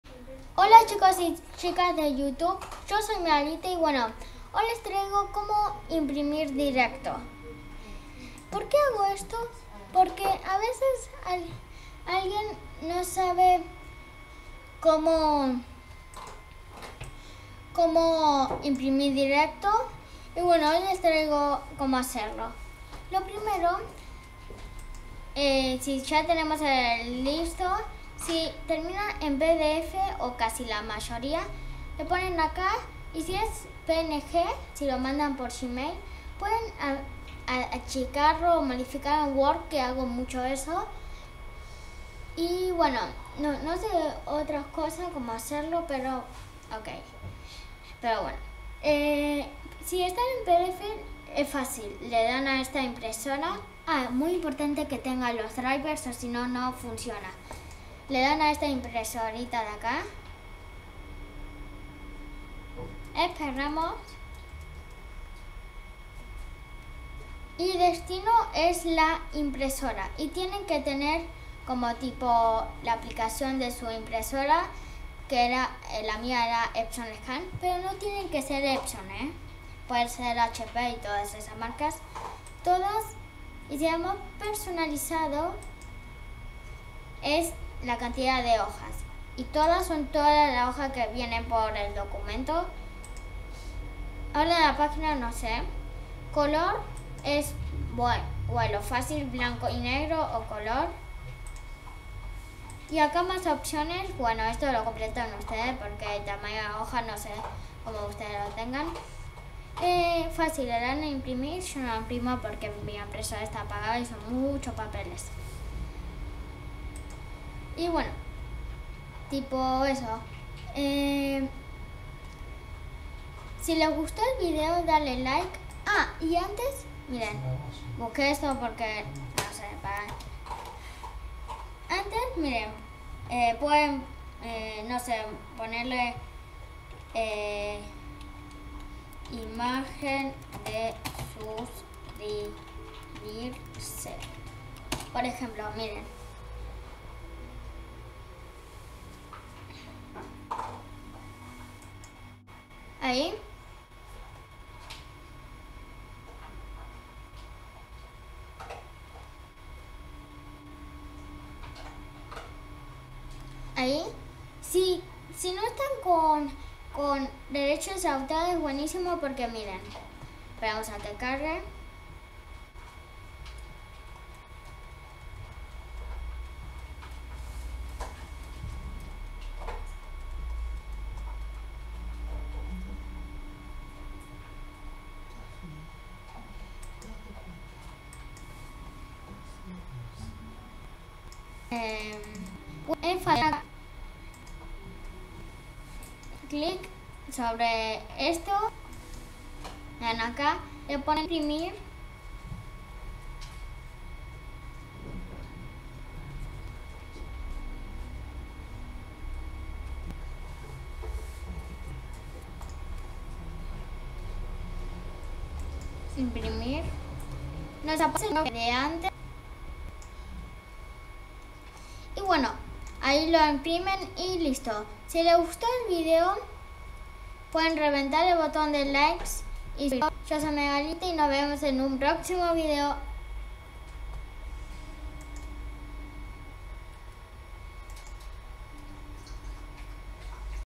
COMO IMPRIMIR DIRECTO EN WINDOWS10
HOLA CHICOS Y CHICAS DE YOUTUBE HOY LES TRAIGO COMO IMPRIMIR DIRECTO PARA WINDOWS10 PARA EL QUIEN NO SABE